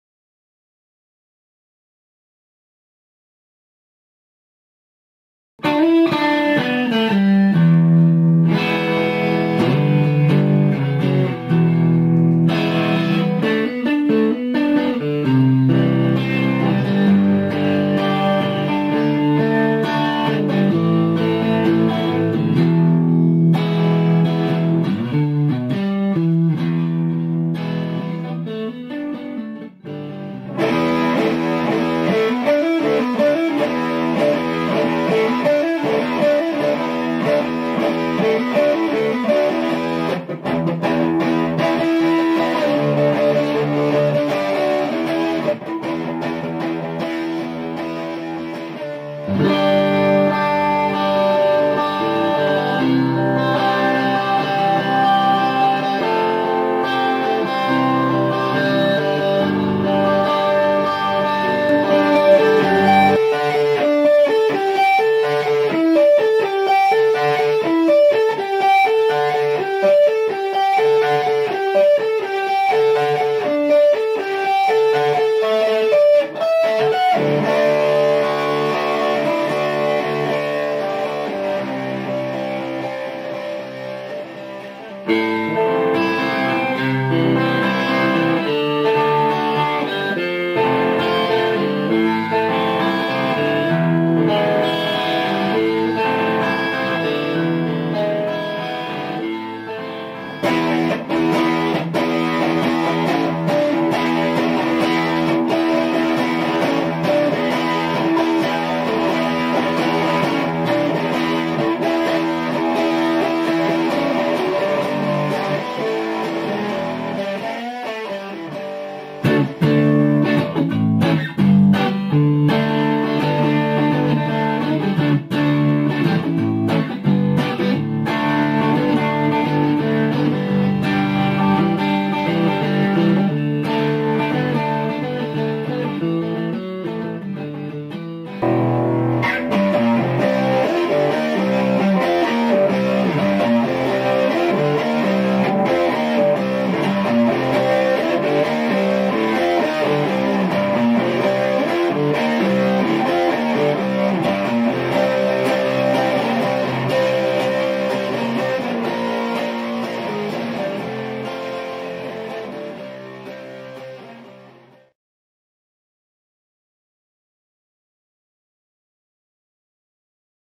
Veranda Jack Danielectro
this guitar was made from the top of a Jack Daniel's Whisky barrel